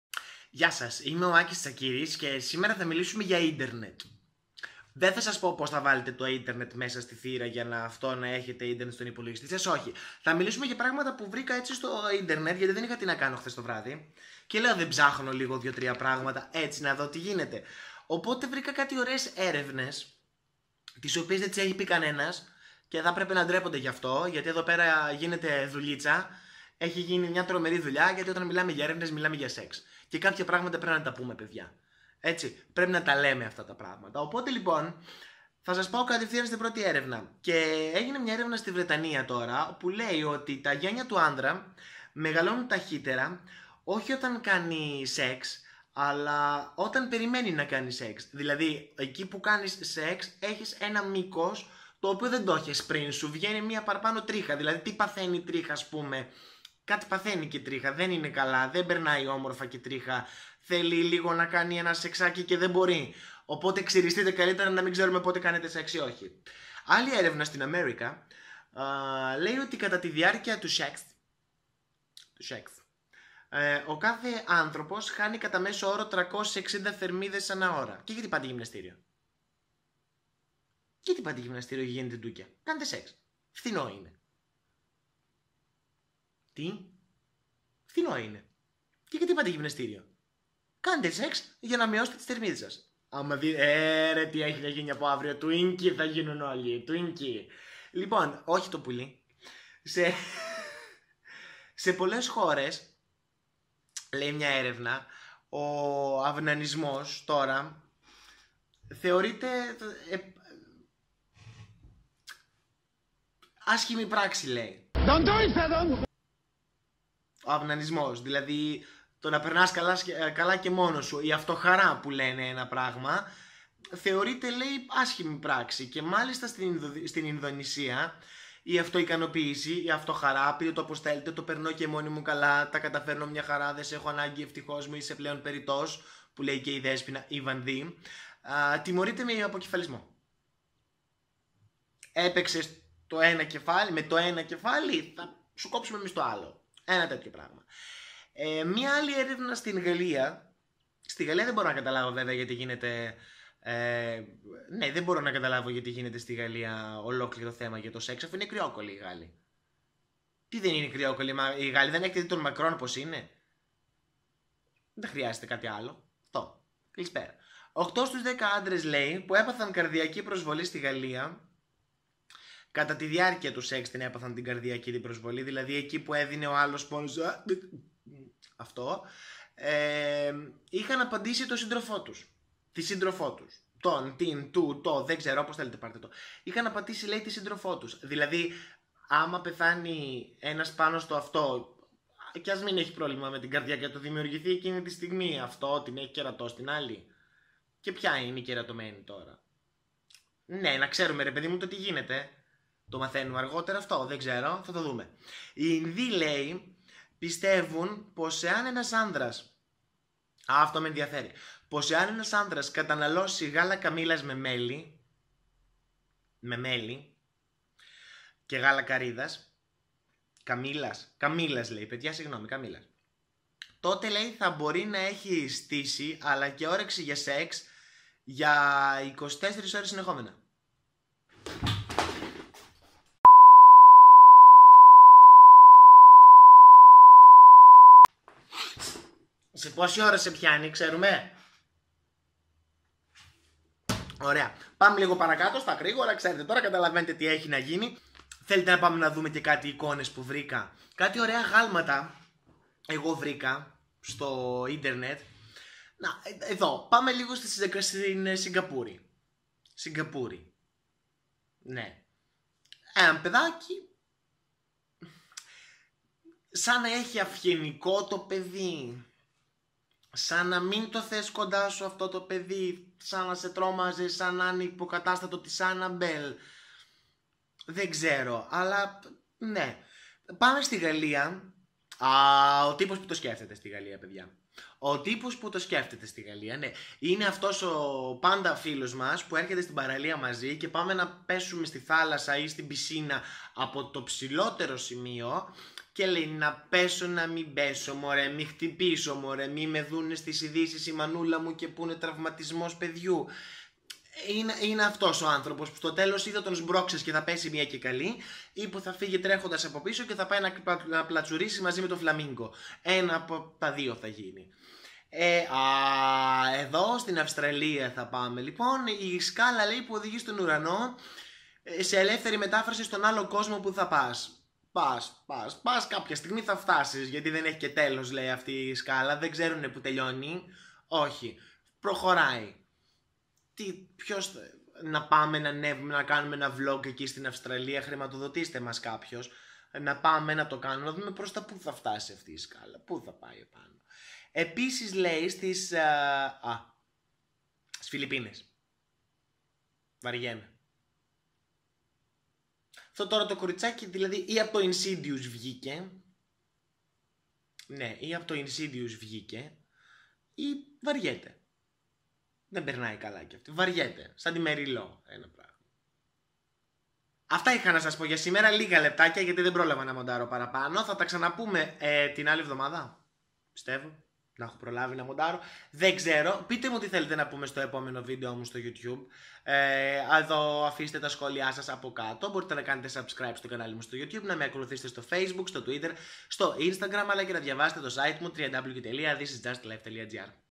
💬Έρευνες από το εξωτερικό και κάτι αγάλματα τριγύρω
Όταν δεν έχεις τί να κάνεις το βράδυ, ψάχνεις στο internet και όταν βρίσκεις διάφορες έρευνες, δεν μπορείς να μην τις σχολιάσεις. Σχολίασε από κάτω όσα σου άρεσαν και όσα δεν σου άρεσαν και σημείωσε τί θέλεις να πούμε στο επόμενο video που θα βγει την επόμενη Τρίτη.